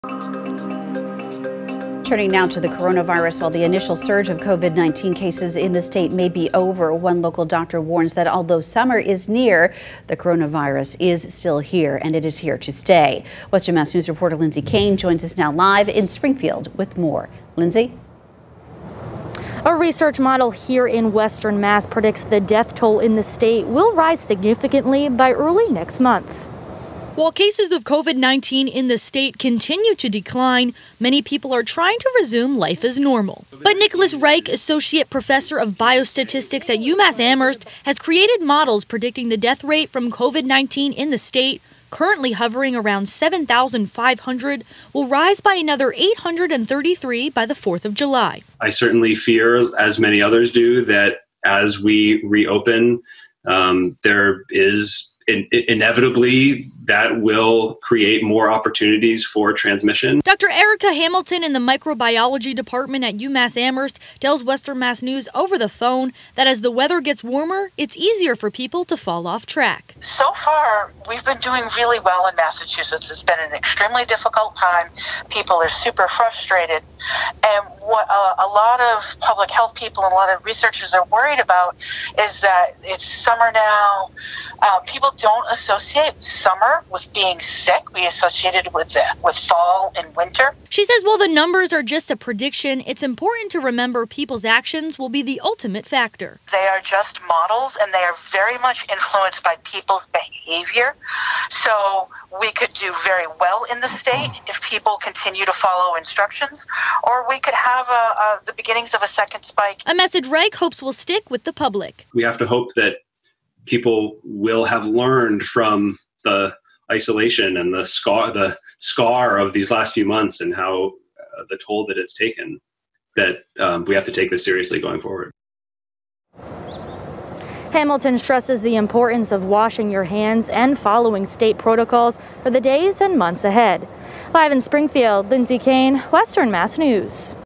UMass experts say COVID-19 deaths could spike by July
A research model here in western Mass predicts the death toll in the state will still rise significantly by early next month.